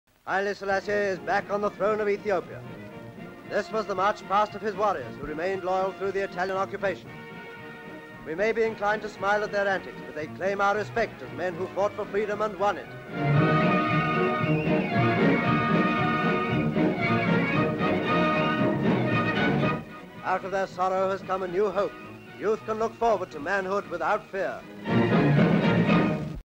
Emperor of Ethiopia Haile Selassie inspects his troops upon his return from exile (1941)
Ethiopian troops march before Emperor Haile Selassie, newly returned to his throne after Italian occupation

Full Description:

ETHIOPIA: Addis Ababa:
EXT

ABYSSINIA Haile Selassie reviews troops outside Palace in Addis Ababa

HAILE SELASSIE Reviews troops outside Palace in Addis Ababa.. chiefs run forward and salute etc..

Emperor Haile Selassie I of Ethiopia; Royalty

East African Campaign, Rastafari movement, WWII, Axis
Background: Ethiopian troops march before Emperor Haile Selassie, newly returned to his throne after Italian occupation